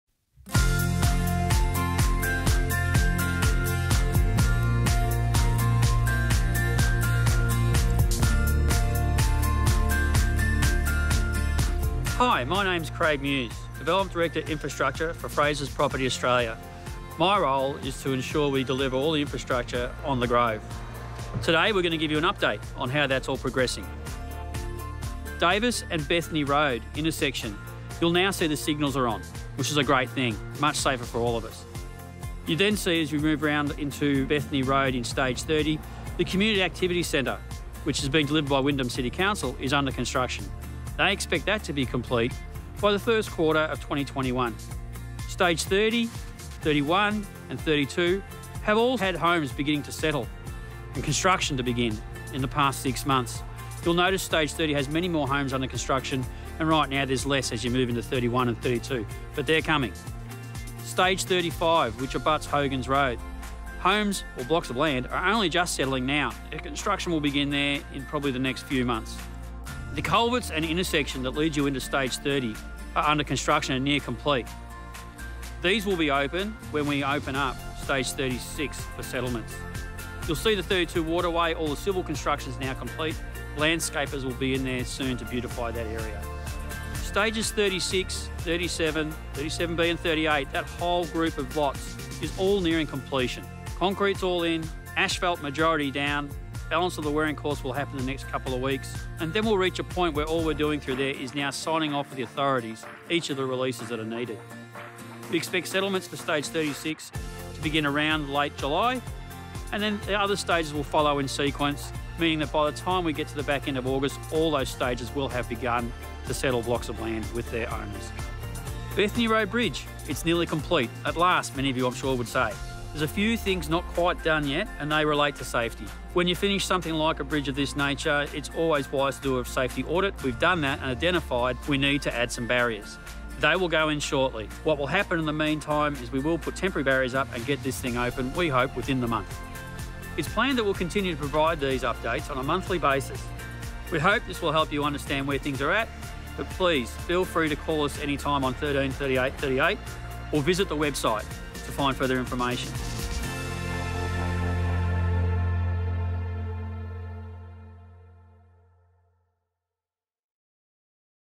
The Grove | Construction Update | June 2020
The Groves' Development Director - Infrastructure, Craig Muse, looks at construction progress across the development, including Bethany Rd Intersection, the Community Activity Centre, Stages 30 - 38,  the waterway and Bethany Rd Bridge.

Enquire today: 13 38 38